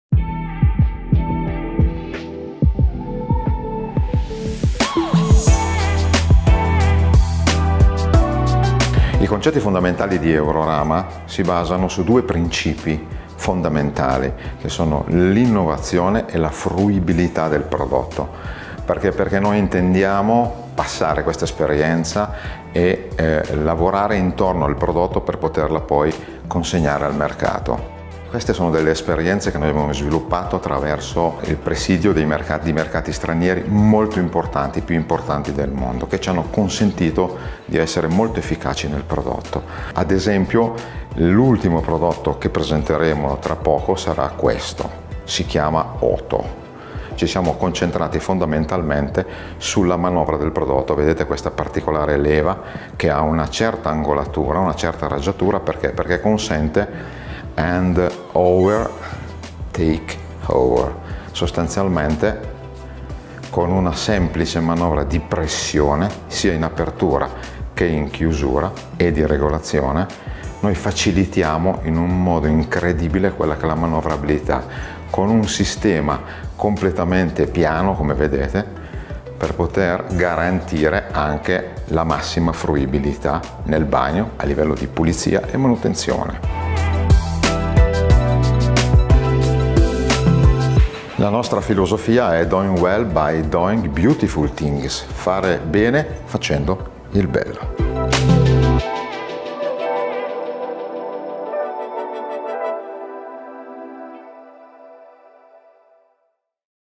EURORAMA SALONE del MOBILE - NEWS 2022
Tante novità in casa Eurorama tra le quali spicca HOTO - Hand Over Take Over. Many news from Eurorama among which HOTO stands out